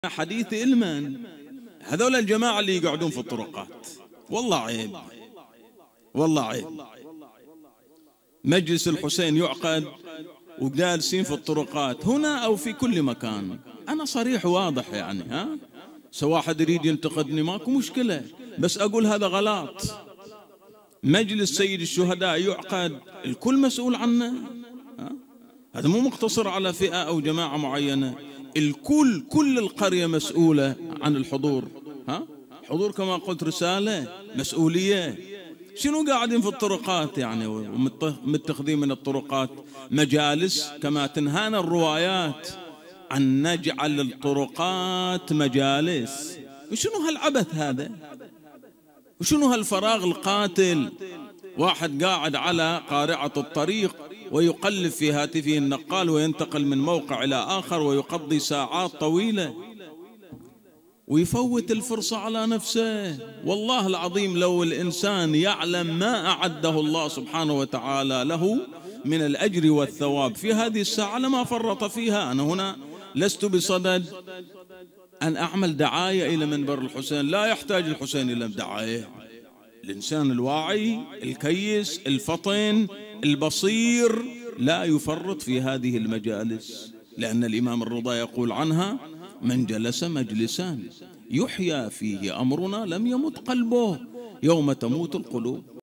ظاهرة الجلوس في الطرقات أثناء قراءة الخطيب في المأتم - الشيخ محمد الريّاش
الشيخ محمد الريّاش - ليلة 1 صفر 1437هـ - مأتم المرخ